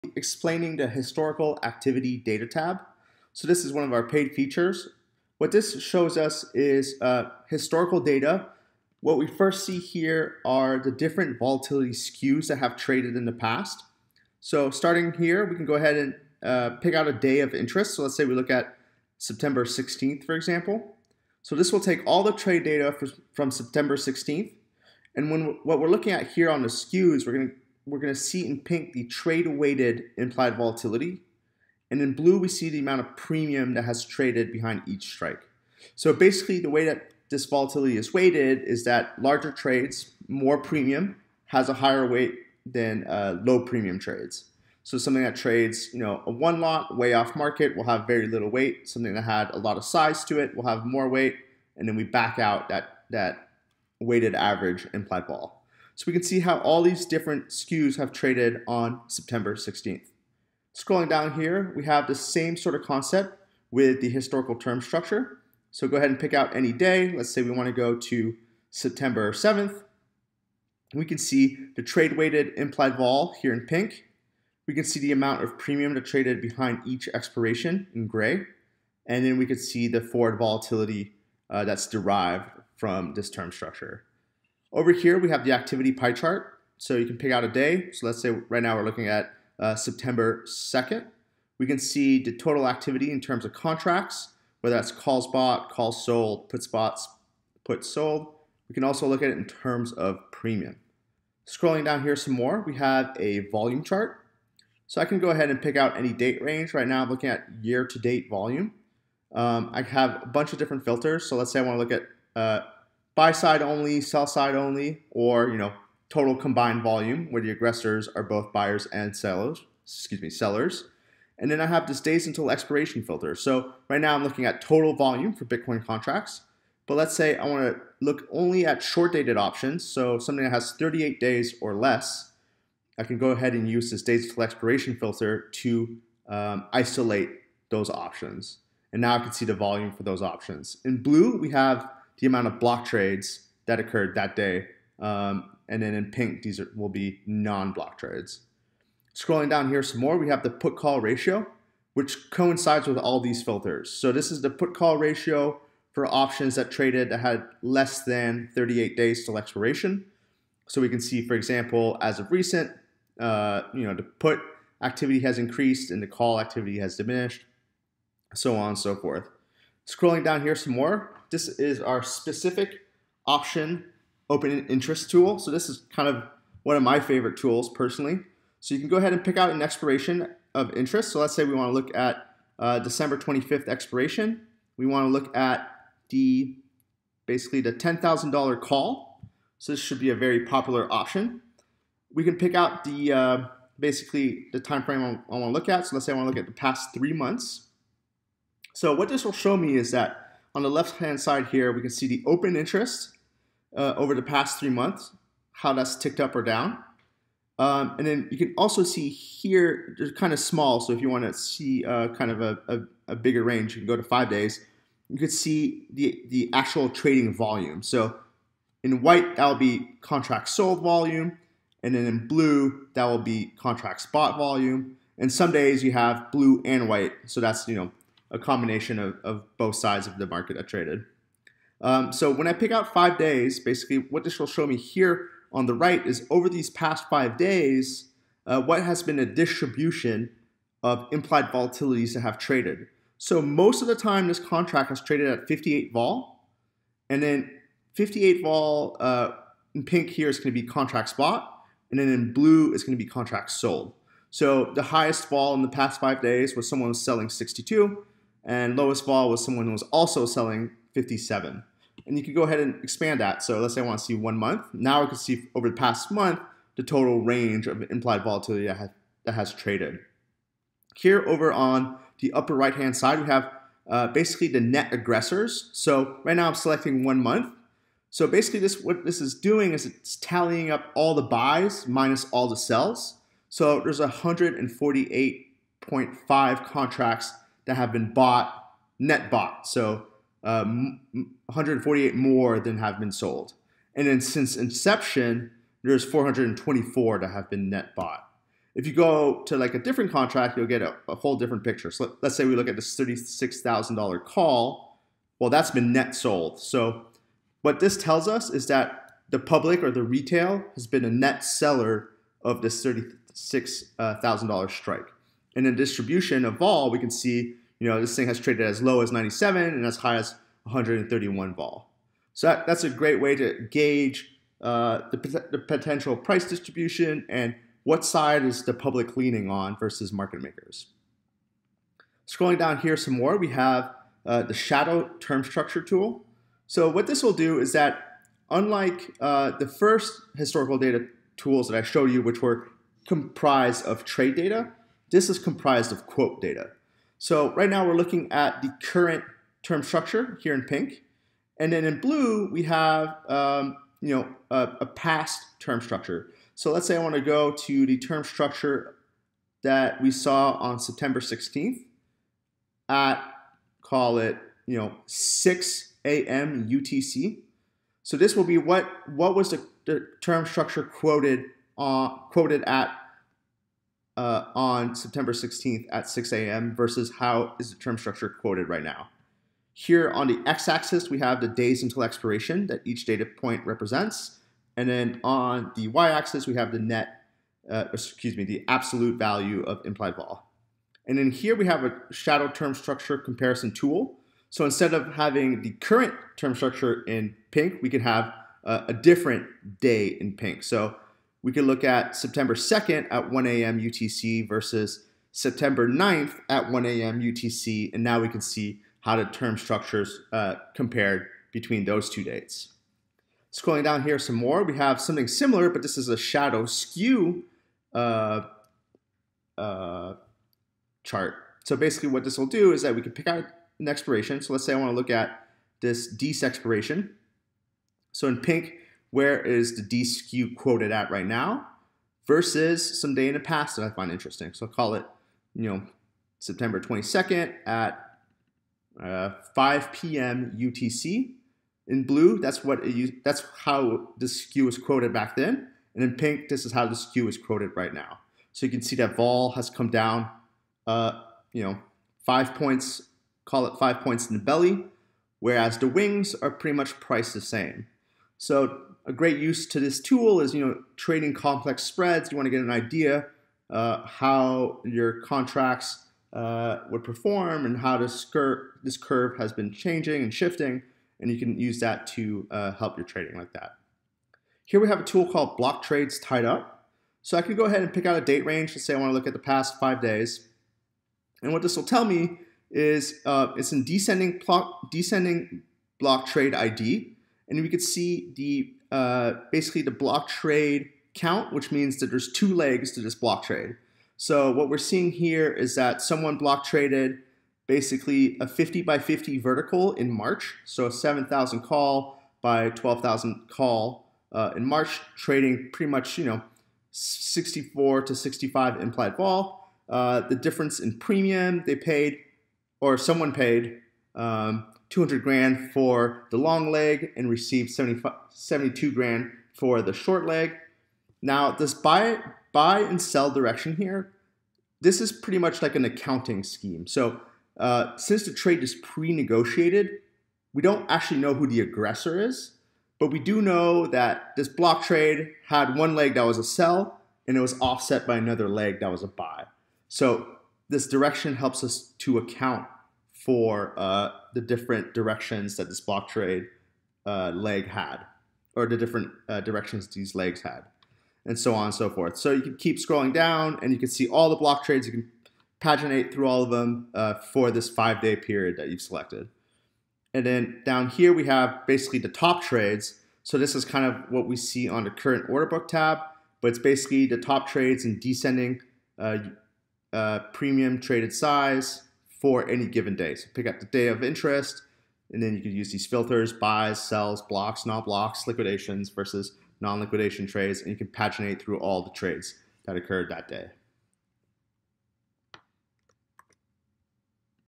Historical Activity
Walk-through for the paid GVOL tools of the "Historical Data Tab" found on gvol.io